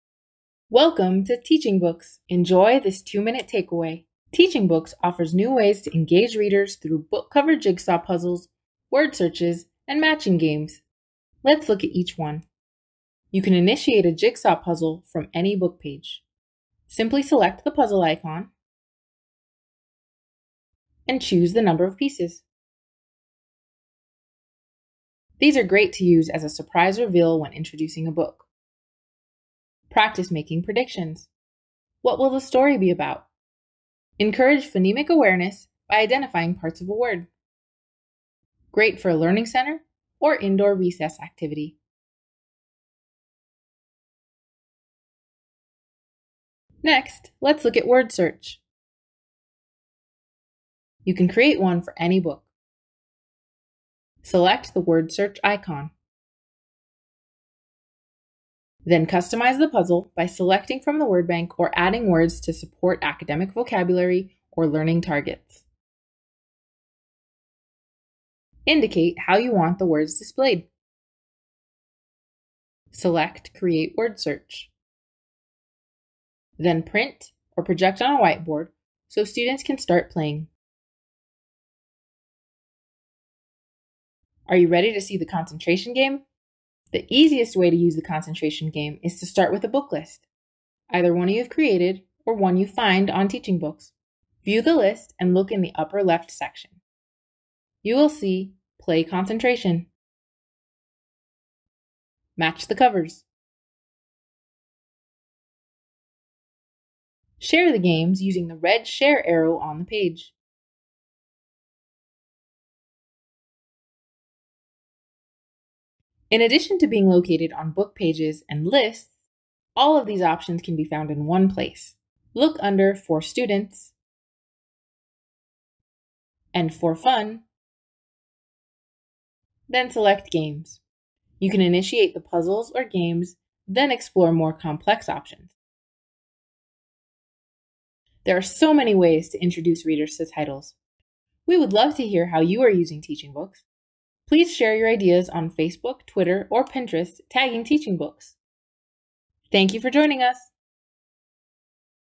Puzzles & Games on TeachingBooks
TeachingBooks offers new ways to engage readers through book cover jigsaw puzzles, word searches, and matching games.